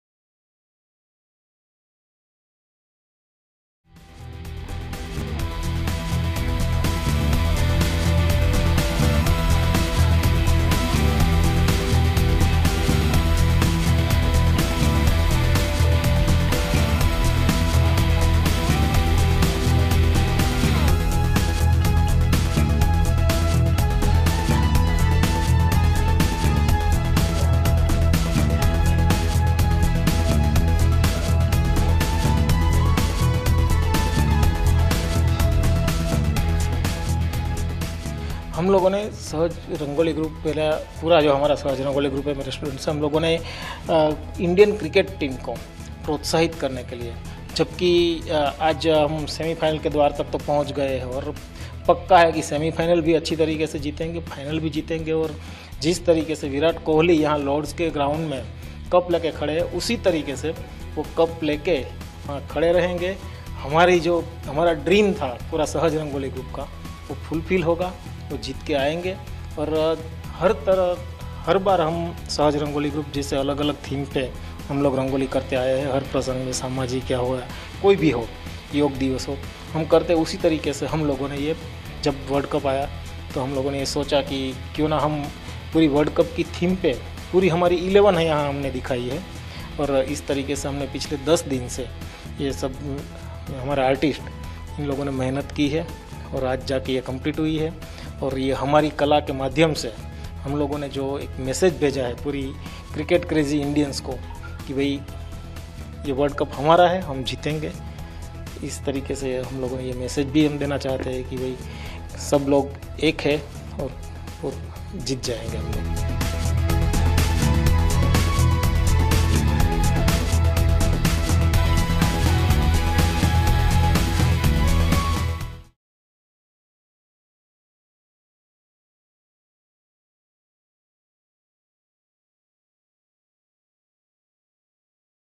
Artists from Gujarat make unique rangolis to motivate Team India
Vadodara (Gujarat): Amid World Cup 2019, artists made rangolis to motivate Indian Cricket Team in Gujarat’s Vadodara.